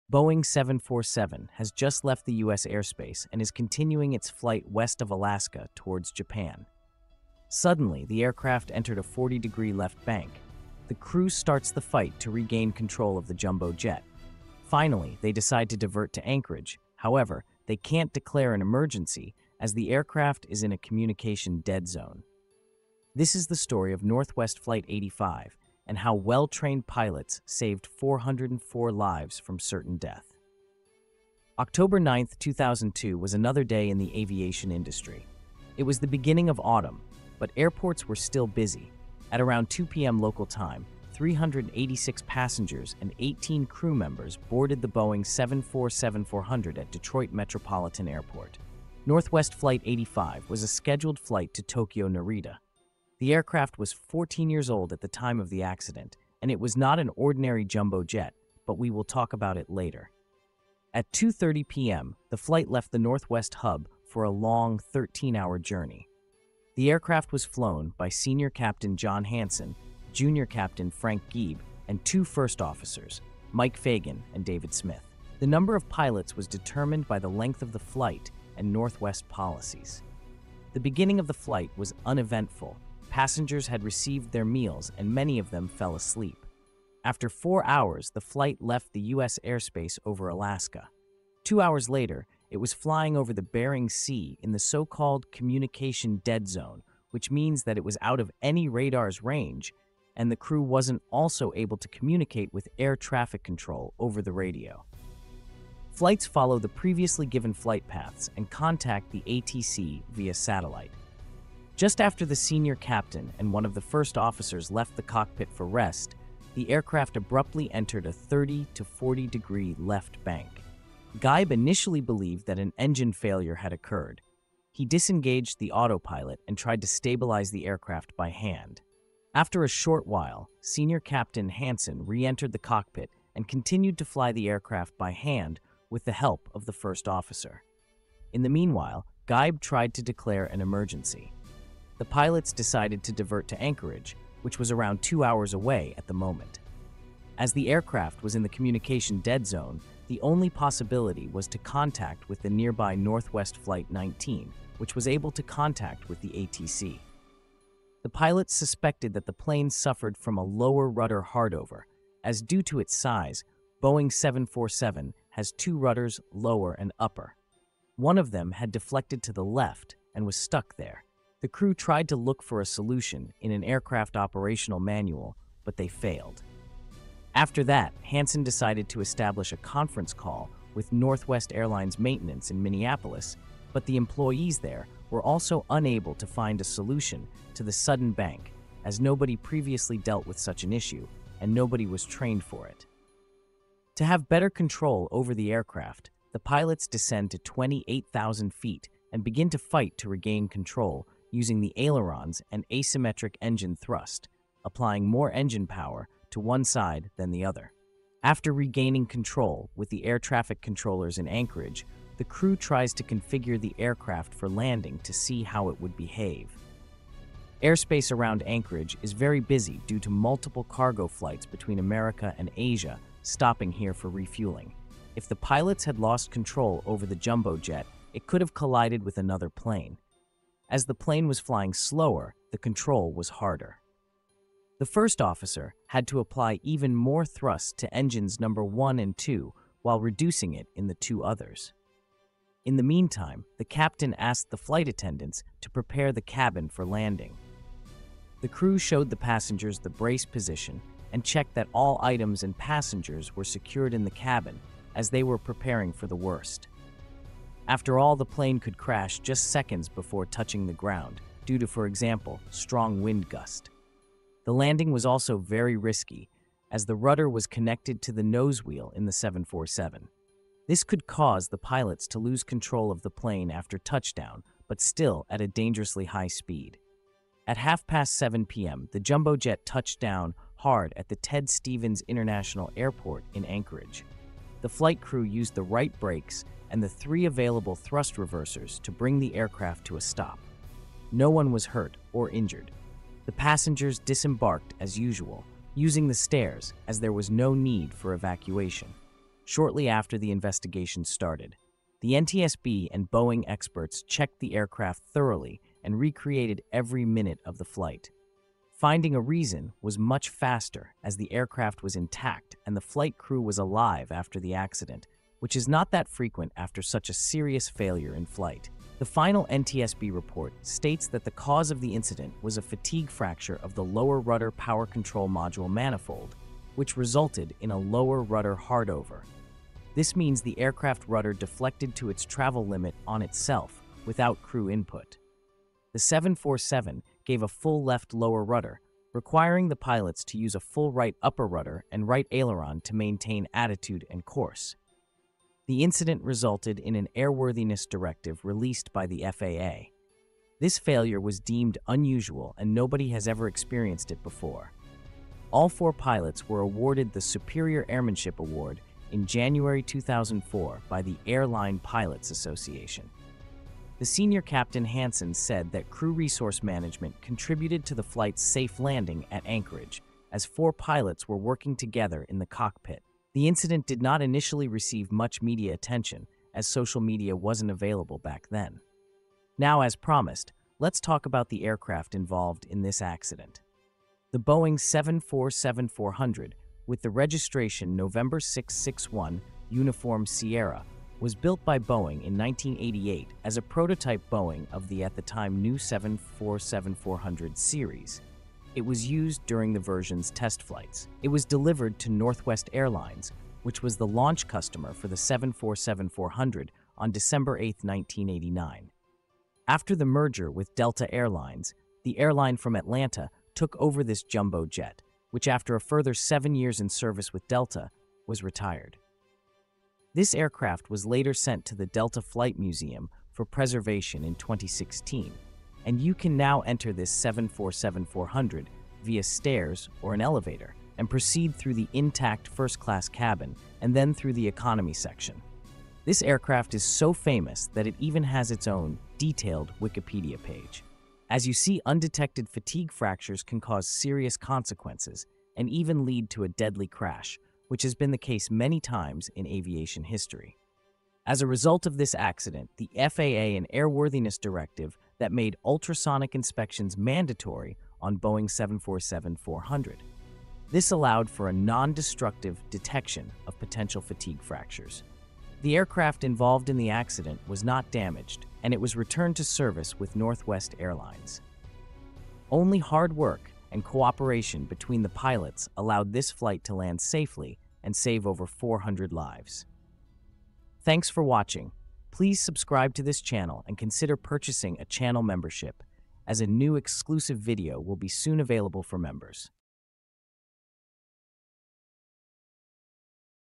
Fight For SURVIVAL! How 4 Pilots SAVED Broken Boeing 747. Northwest Airlines Flight 85
Northwest Flight 85, a Boeing 747-400 flying from Detroit to Tokyo, faced a terrifying situation over the Bering Sea when it suddenly veered left due to a rudder failure. Discover how four skilled pilots fought to regain control of the aircraft in a communication dead zone, ultimately saving 404 lives. Learn about the NTSB investigation, the cause of the rudder hardover, and the heroic landing at Anchorage. Explore this real-life aviation incident, the role of crew resource management, and the Boeing 747's history in this gripping story of a near-tragic flight.